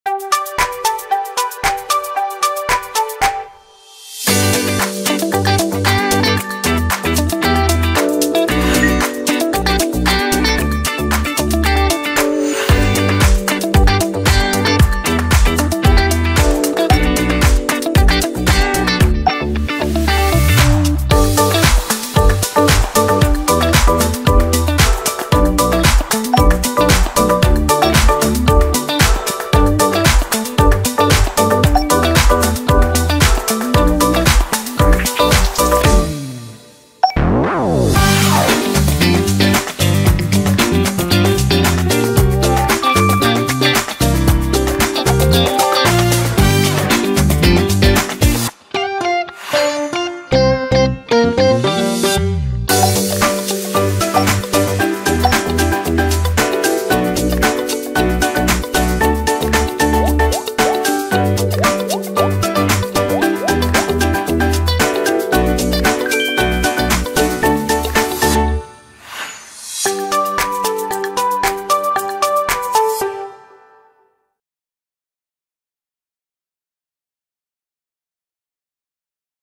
Galaxy Watch6 | Watch6 Classic: How to track your sleep | Samsung
Start your day off with more energy by monitoring your sleep on the Galaxy Watch6 Series. See how you can get detailed sleep coaching to improve your sleep habits.